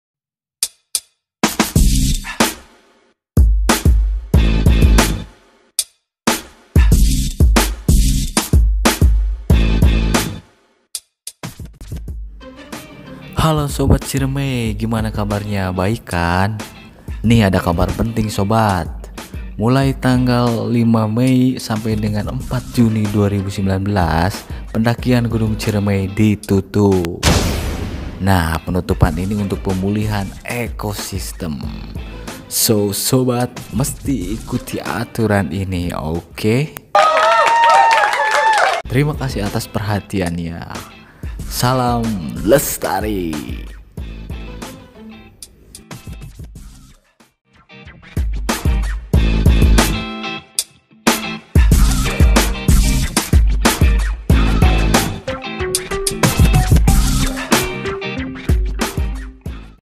Pendakian Gunung Ciremai Ditutup
5 Mei - 4 Juni 2019 pendakian gunung Ciremai ditutup dan dibuka kembali 5 Juni 2019